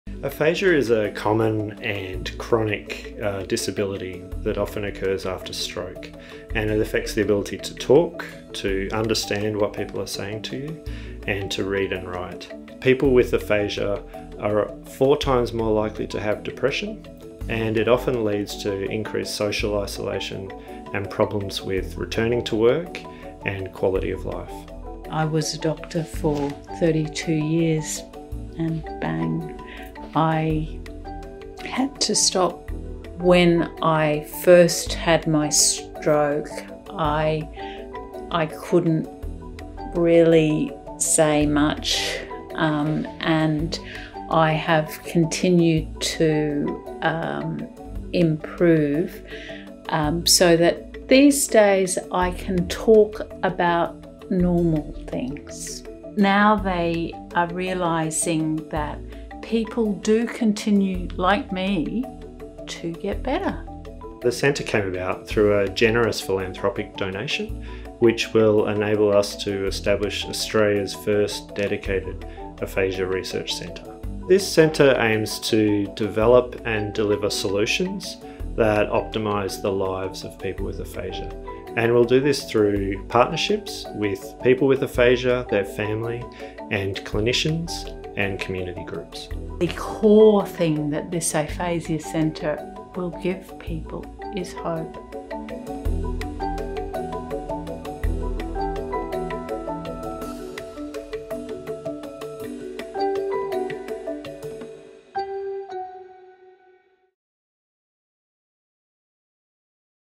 Professor David Copland talks about Australia's first dedicated aphasia research centre
Aphasia is a chronic and common disability that often occurs after stroke, and affects approximately 140,000 people in Australia. Thanks to generous philanthropic support, UQ is set to open Australia’s first dedicated aphasia research centre. Hear from Professor David Copland on the establishment of the centre and the impact this will have for people with aphasia.